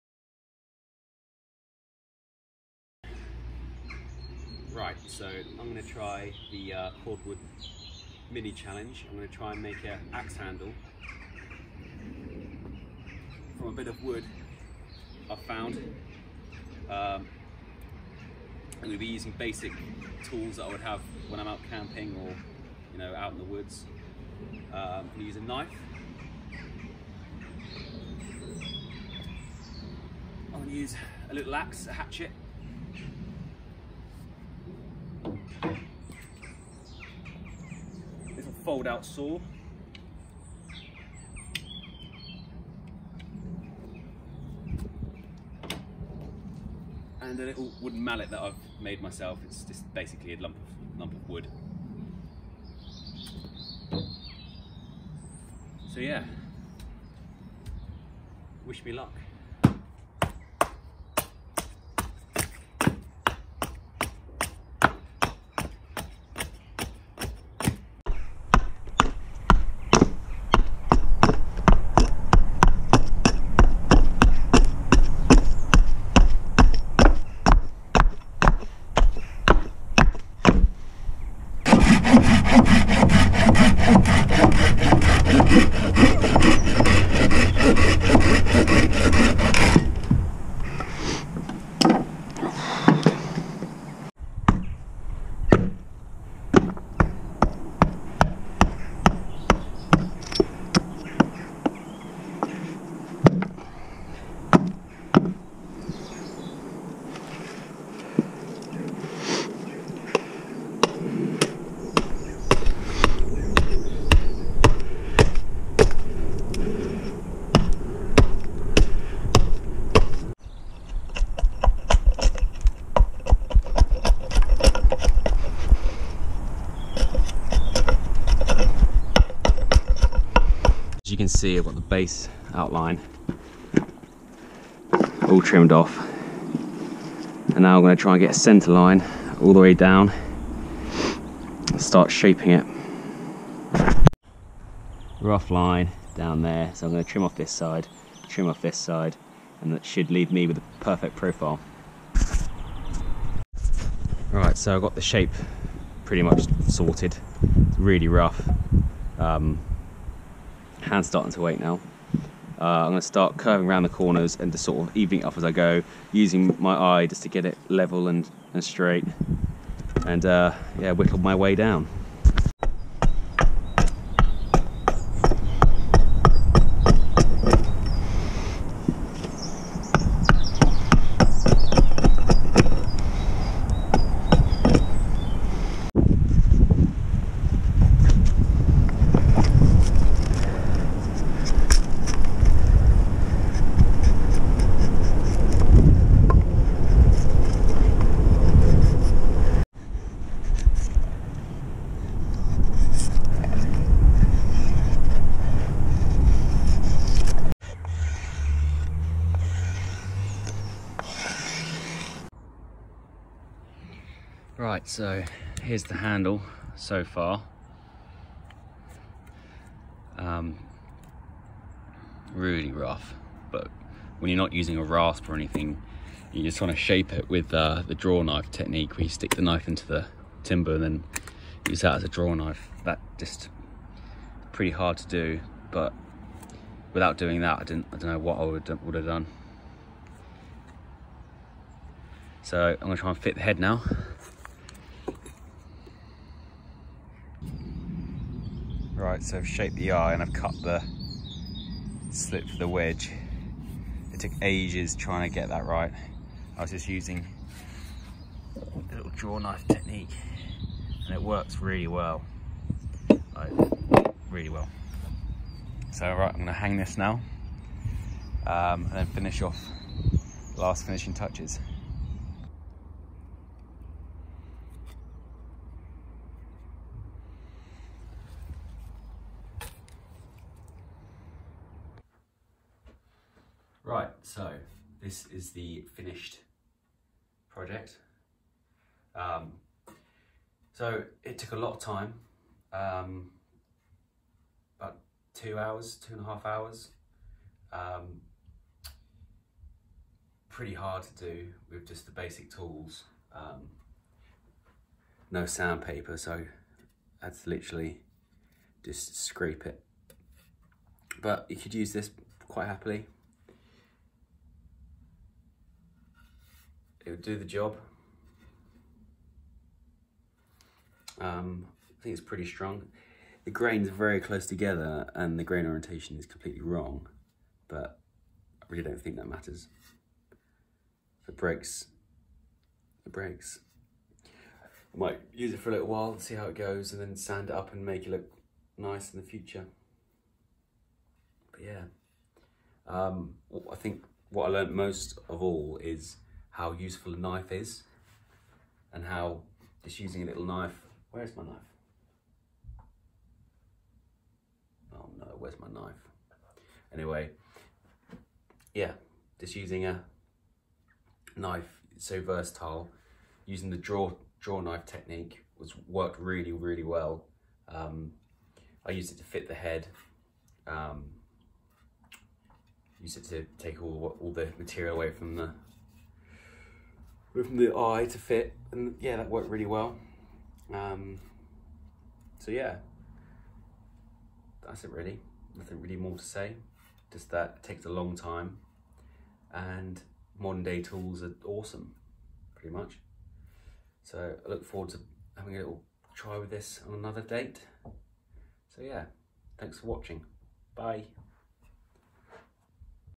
Making an axe handle in the field with only hand tools (axe cordwood challenge)
Using a locally sourced piece of ash I found in the woods in South Wales I carve and whittle an axe handle from scratch only using a hatchet, knife and small saw. I leant that the knife is the most useful tool and can never be underestimated. The axe head itself is a modded elwell made into a Hudson Bay pattern, this style of axe was used buy trappers in Canada back in the day. The pattern has a very shallow eye which made fitting a lot quicker and easier (maybe this is a reason they chose this style of axe)